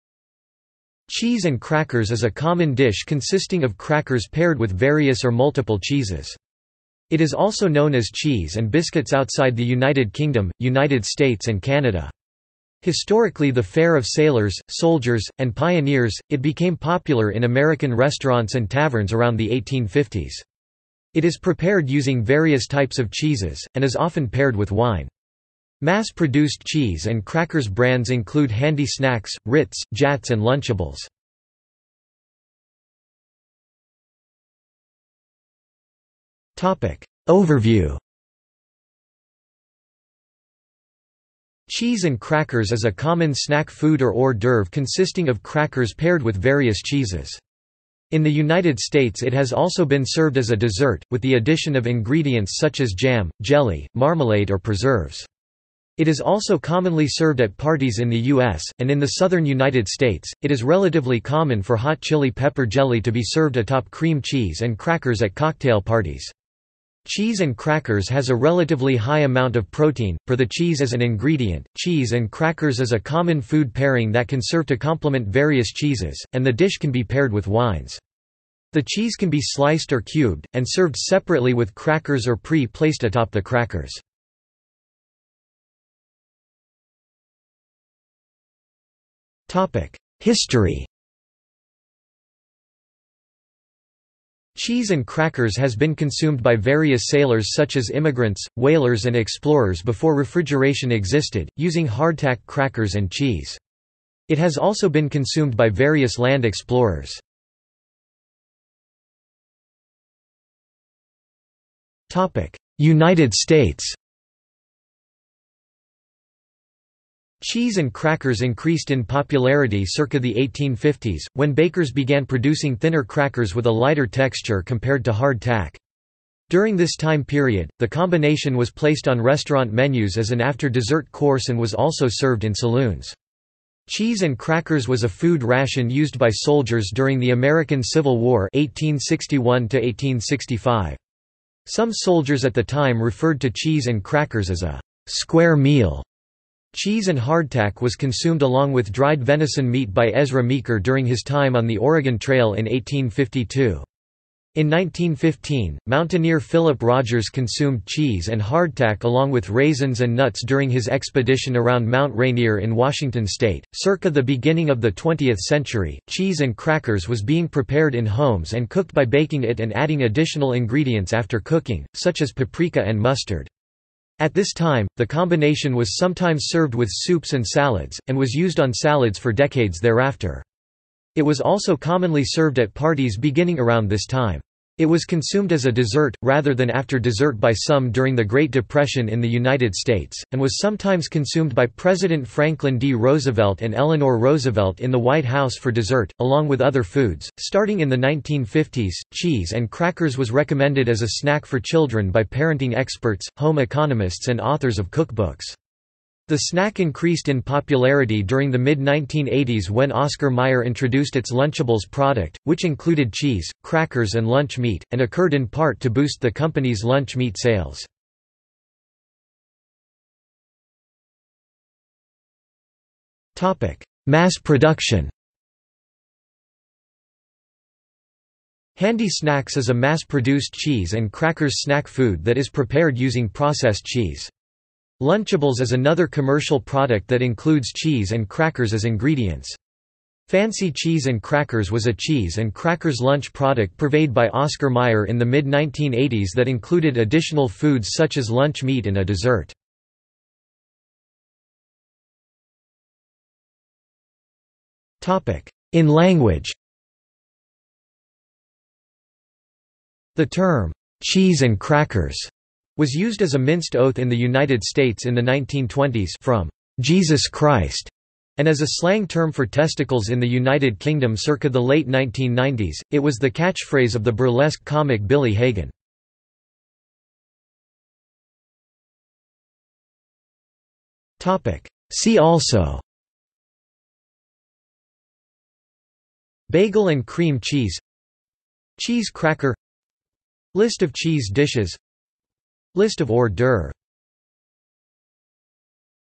Handi-Snacks | Wikipedia audio article
This is an audio version of the Wikipedia Article:
Handi-Snacks

00:00:33 1 Overview
00:01:25 2 History
00:01:48 2.1 United States
00:03:39 3 Mass production
00:04:11 4 In language
00:04:37 5 See also

- Socrates

SUMMARY
Cheese and crackers is a common dish consisting of crackers paired with various or multiple cheeses. It is also known as cheese and biscuits outside the United Kingdom, United States and Canada. Historically the fare of sailors, soldiers, and pioneers, it became popular in American restaurants and taverns around the 1850s. It is prepared using various types of cheeses, and is often paired with wine. Mass-produced cheese and crackers brands include Handi-Snacks, Ritz, Jatz and Lunchables.